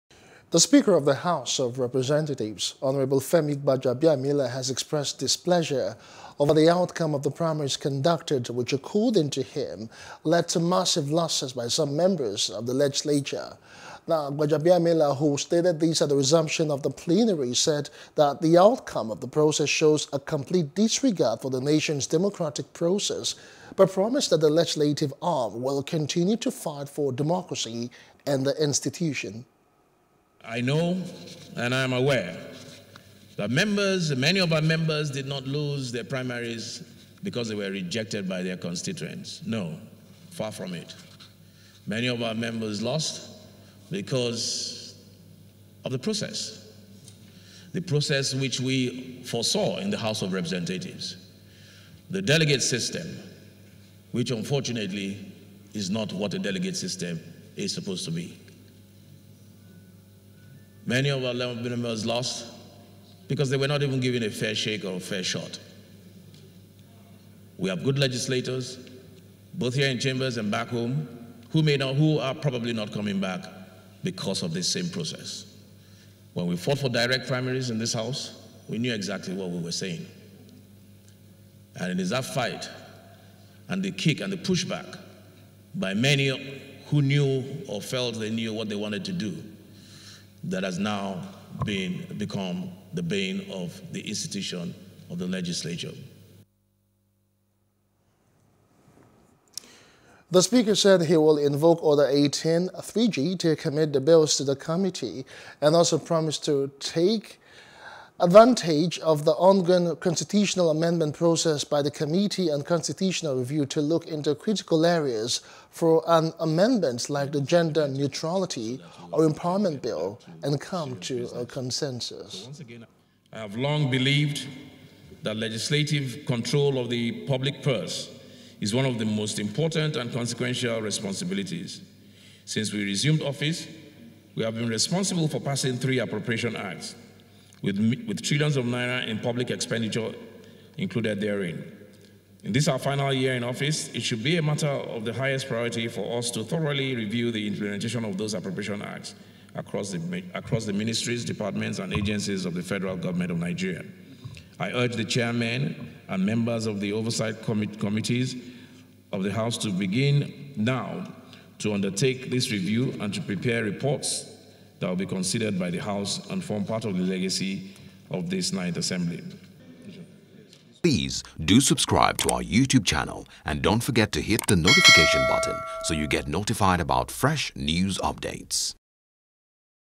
NASS Resumption: Speaker Faults Primaries, Plans To Invoke Order 18,3g On Unattended Bills | NEWS
The Speaker of the House of Representatives Hon. Femi Gbajabiamila has expressed displeasure over the outcome of the primaries conducted, which according to him, led to massive losses by some members of the legislature.